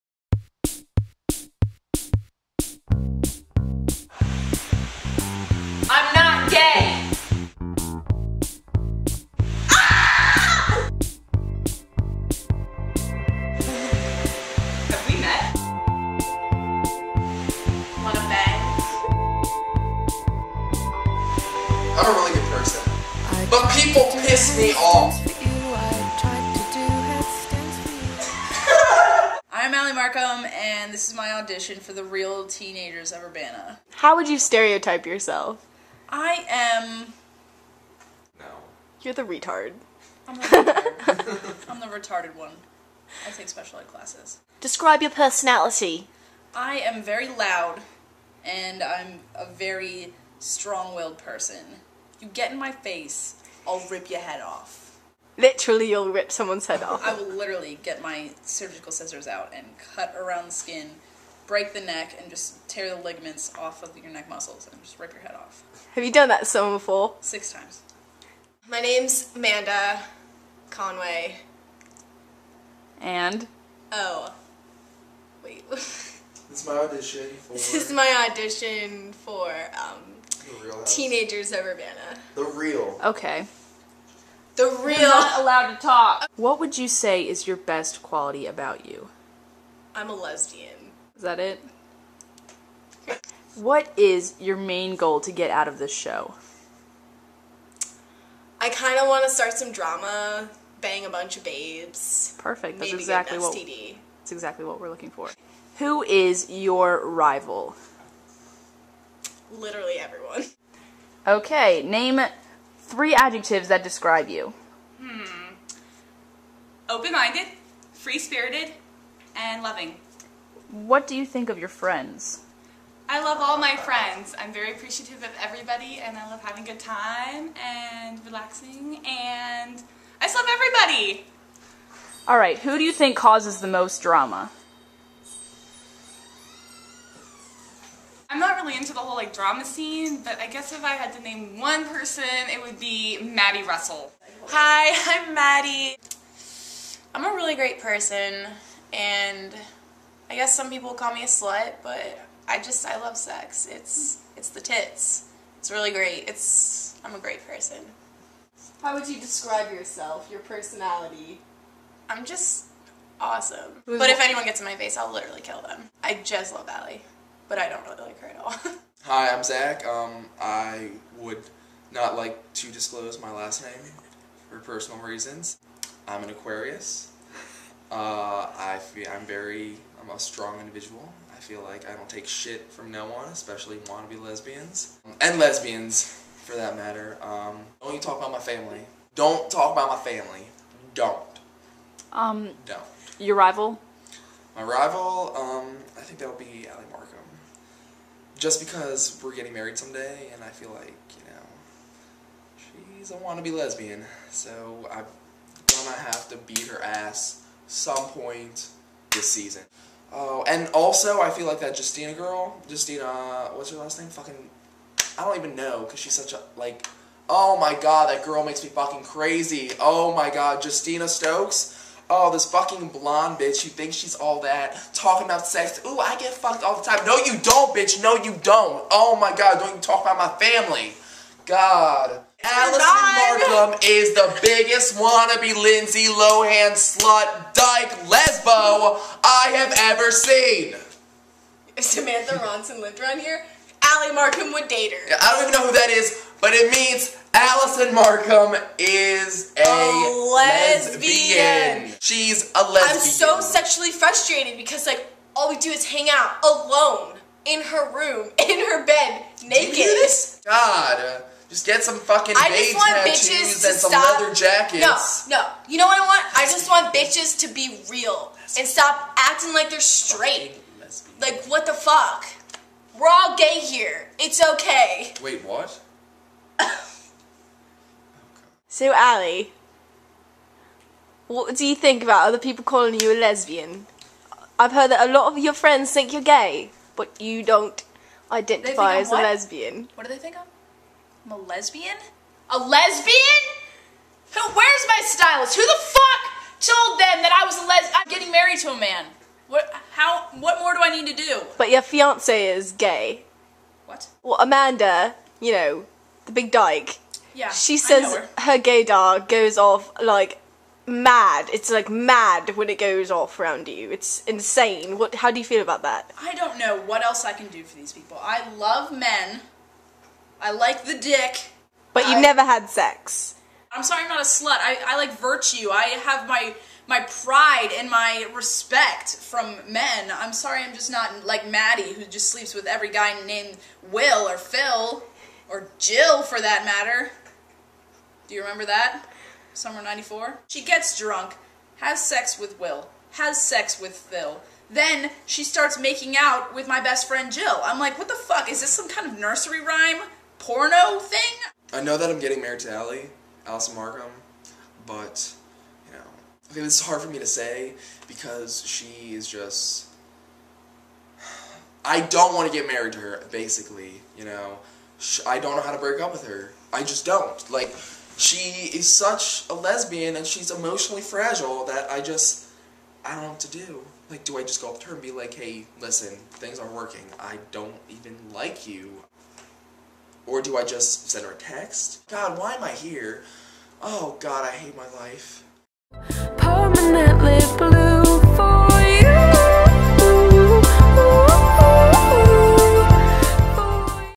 The Real Teenagers of Urbana: Part One
A parody of The Real Housewives of New Jersey/every "reality" show out there. The confessionals and interviews are parodied in this section. Stick around for more! It'll be coming soon! That's what she said.